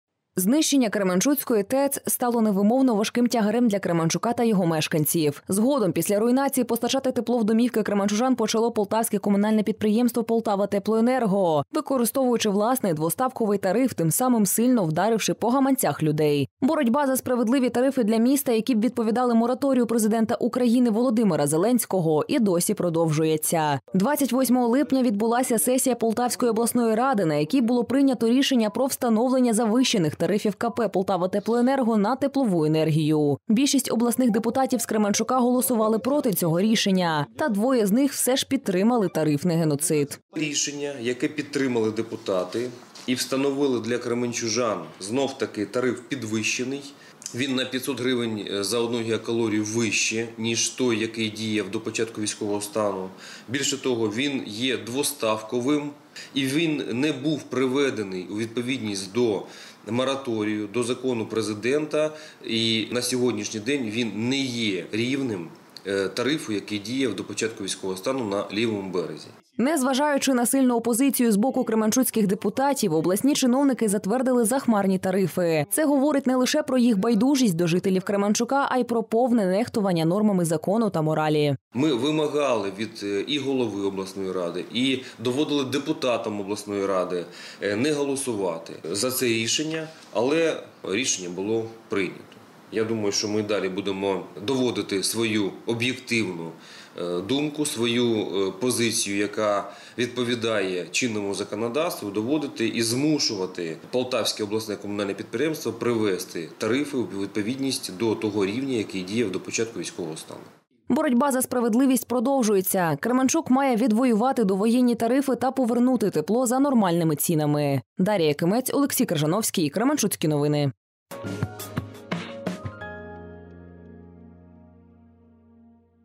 28 липня на сесії обласної ради, було прийнято рішення про встановлення завищених тарифів на тепло
28 липня відбулася сесія Полтавської обласної ради, на якій було прийнято рішення про встановлення завищених тарифів КП «Полтаватеплоенерго» на теплову енергію